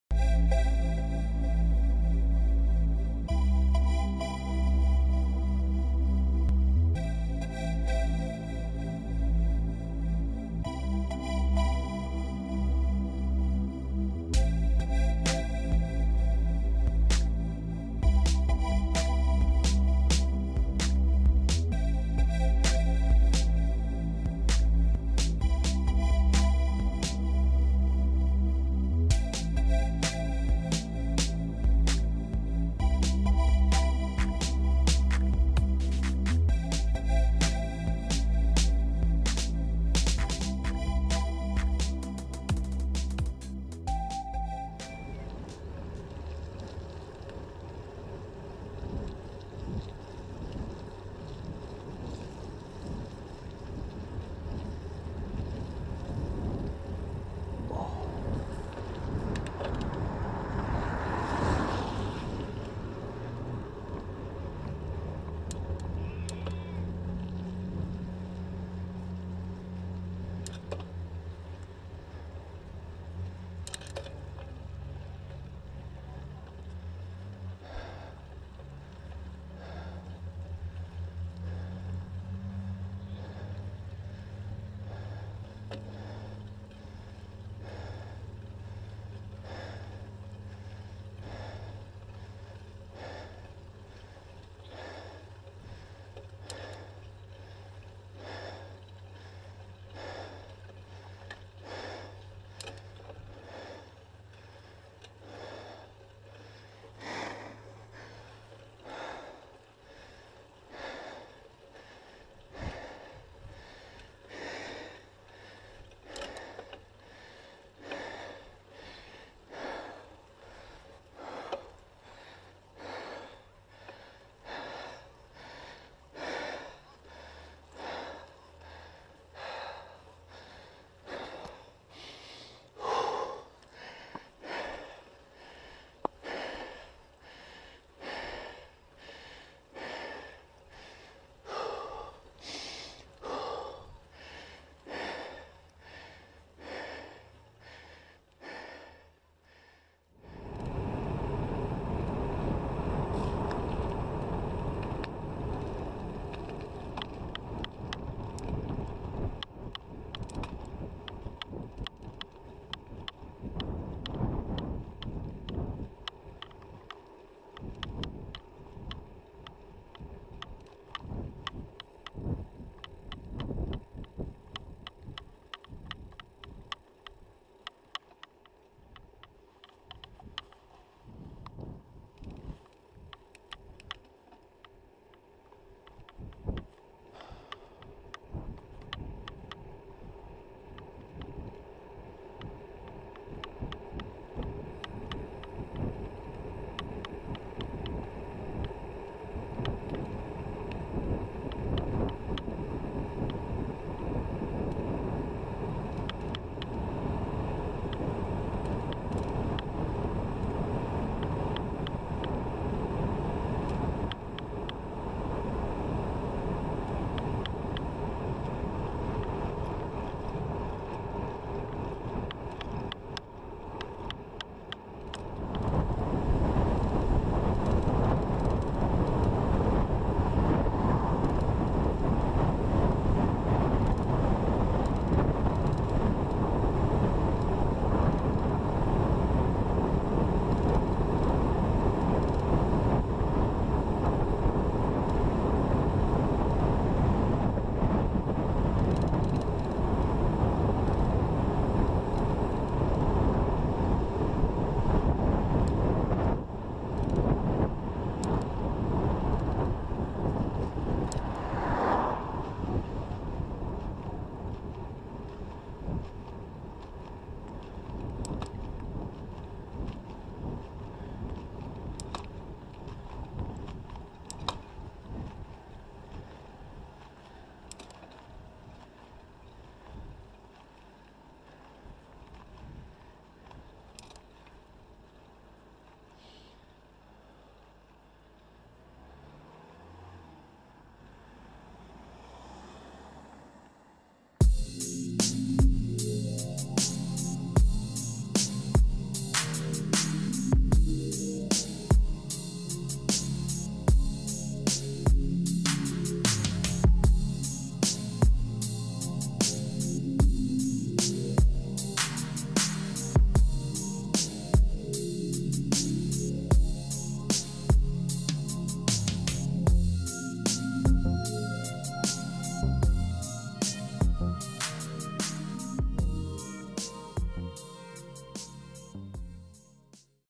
Trek Duo Sport 4 2021 Hill & Road Test
A short montage riding the DS4 in north Yorkshire England.